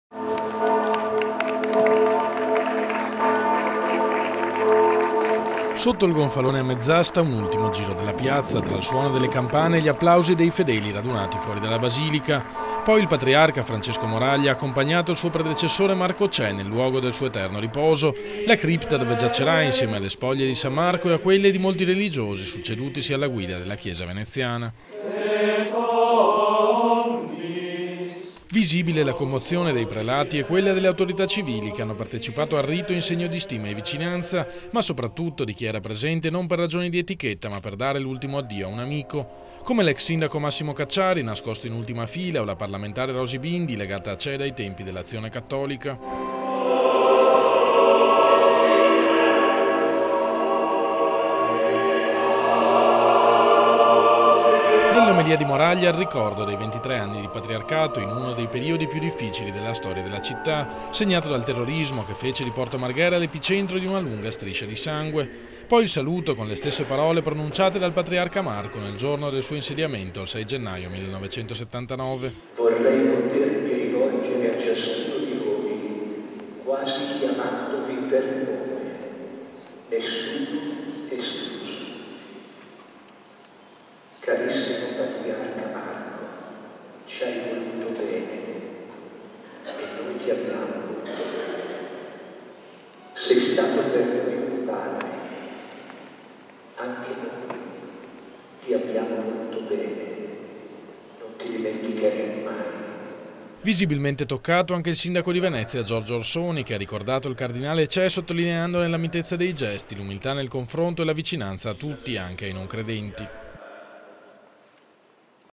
17/05/2014 - IL PATRIARCA CE' SEPOLTO ACCANTO A SAN MARCO
Nella basilica di San Marco la cerimonia funebre per il cardinale Marco Cè, già Patriarca di Venezia, morto qualche giorno fa all'età di 88 anni.